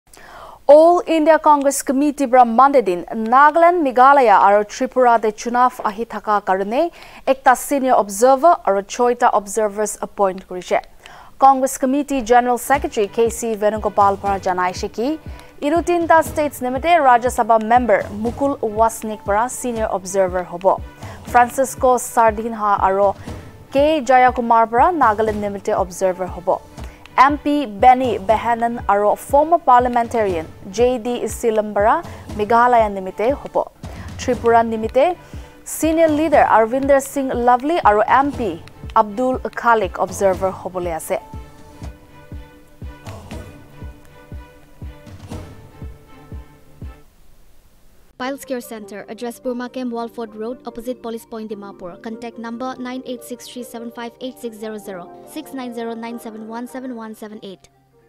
CONGRESS APPOINTS OBSERVERS FOR ASSEMBLY POLLS 2023 IN NAGALAND, MEGHALAYA & TRIPURA
All India Congress Committee on 26th December appointed one senior observer and 6 observers for the upcoming assembly elections in Nagaland, Meghalaya and Tripura. Congress Committee general secretary KC Venugopal announced that Rajya Sabha member Mukul Wasnik will be senior observer for the 3 states. For Meghalaya, MP Benny Behanan and former parliamentarian JD Seelam would be the observers while Francisco Sardinha and K Jayakumar for Nagaland. Meanwhile, senior leader Arvinder Singh Lovely along with MP Abdul Khaleque would be the observers for Tripura